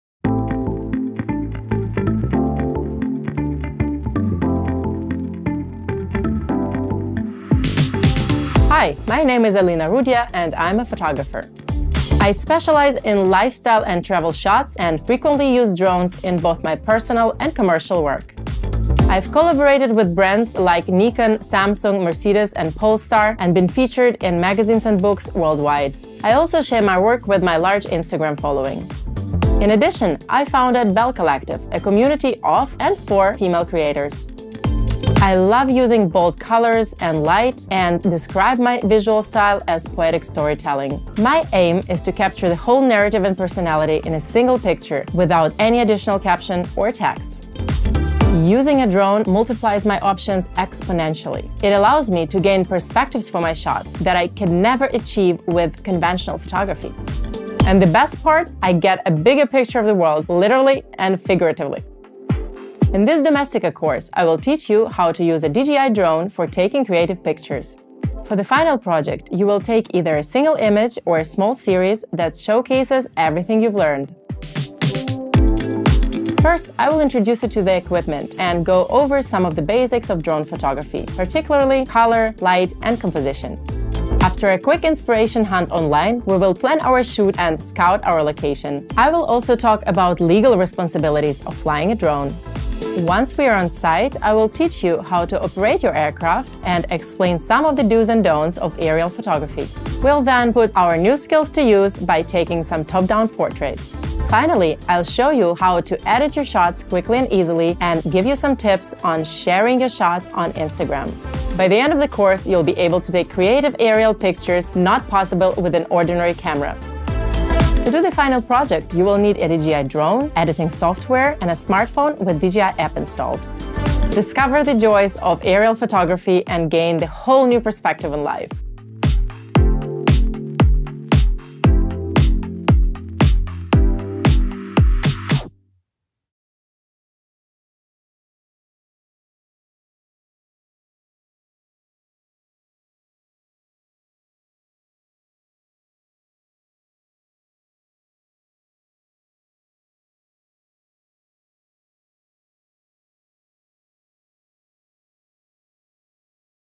DRONE Photography for Beginners: Capture the Bigger Picture - Course by Alina Rudya | Domestika
Learn how to take and edit captivating aerial photographs that combine light, composition, color, and perspective

Using a drone to take photos multiplies your options exponentially. it allows you to gain perspectives that aren’t possible with conventional photography and get a bigger picture of the world - both figuratively and literally. Photographer Alina Rudya uses drones as part of her creative process. She has had her work published internationally and worked with clients including Nikon, Mercedes-Benz, and Samsung.

In this online course, Alina teaches you how to use a DJI drone for taking creative pictures. Explore light, composition, color, and perspective to capture creative pictures not possible with an ordinary camera. Share your unique view of the world with aerial photography!